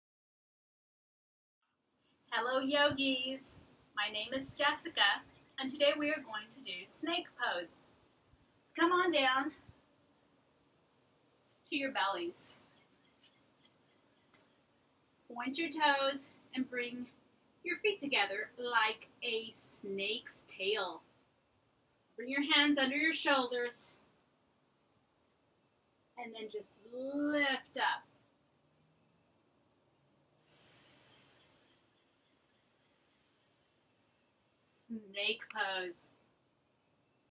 Kids Yoga: Snake Pose
Hisssss...Snake time! Practice being a Sssssnake with Ms. Jessica!

Snake pose stretches your spine, back and spine muscles, and strengthens your spine and shoulders. Opens your heart and lungs and elevates your mood!